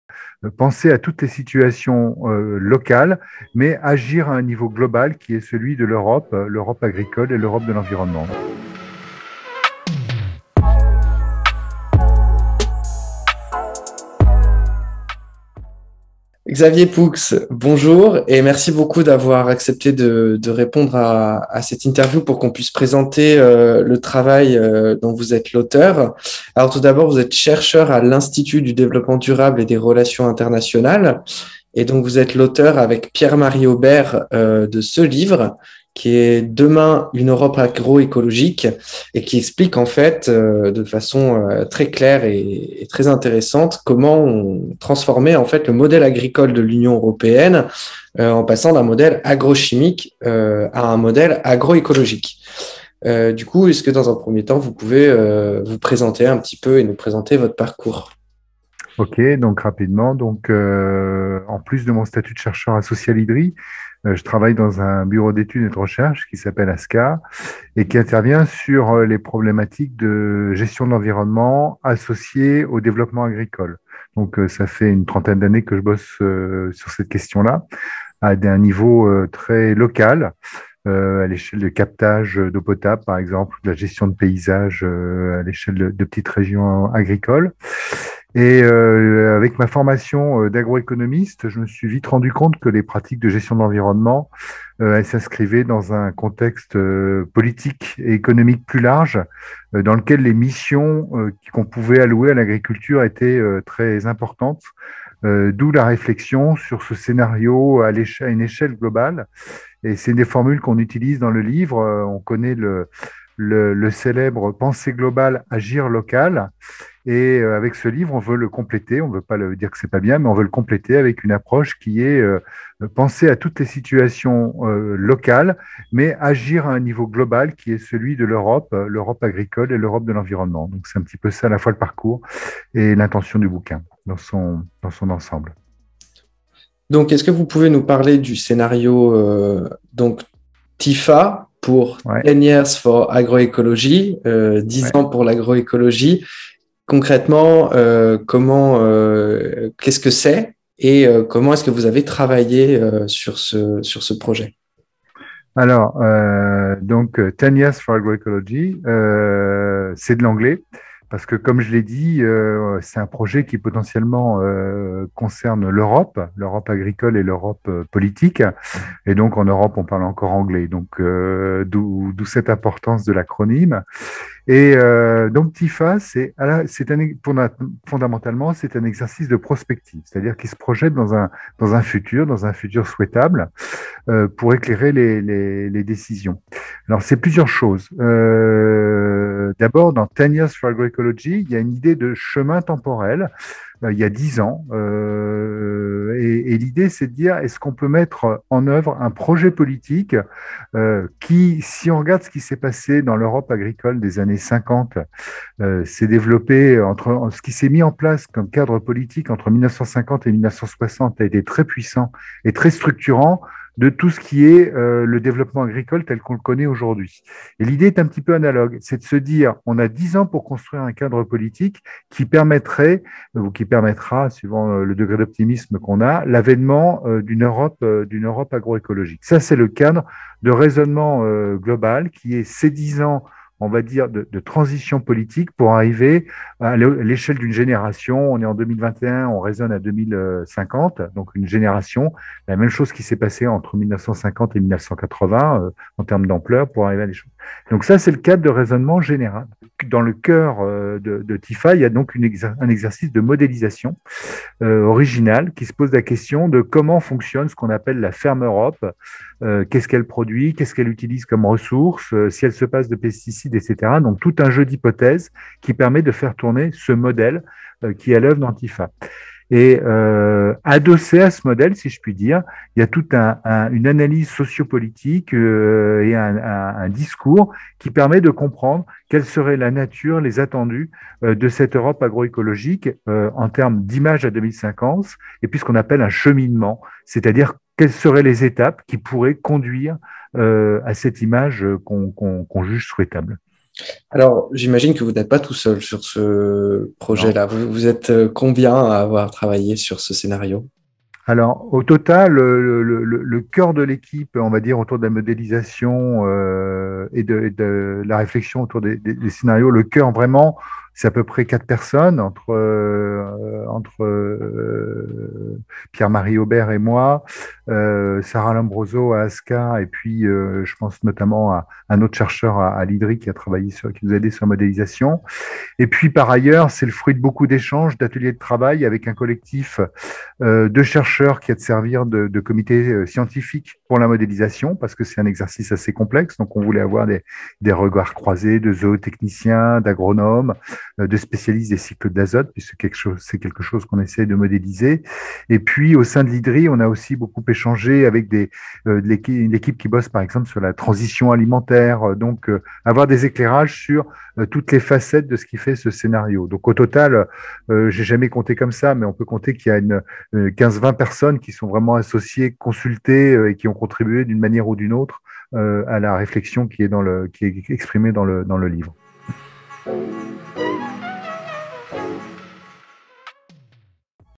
Vers une Europe agroécologique : Épisode 01 – Le scénario dix ans pour l’agroécologie (TYFA)
🍃 Secrets Toxiques se bat pour la généralisation d’un modèle alimentaire agroécologique, qui se passe de pesticides à la toxicité sous-évaluée. Le livre de Xavier Poux et Pierre-Marie Aubert Demain, Une Europe Agroécologique nous montre que cette agriculture n’est pas une utopie, et qu’elle est à notre portée.
Pour en savoir plus, retrouvez notre entretien avec Xavier Poux en six épisodes, répondant chacun à une des questions essentielles sur les conséquences sur notre alimentation d’un modèle agroécologique.

📽️ Dans l’épisode d’aujourd’hui, Xavier Poux nous présente le scénario Dix Ans Pour l’Agroécologie, sa logique et sa méthode.

👉 Vous pouvez également commander le livre de Xavier Poux et Pierre-Marie Aubert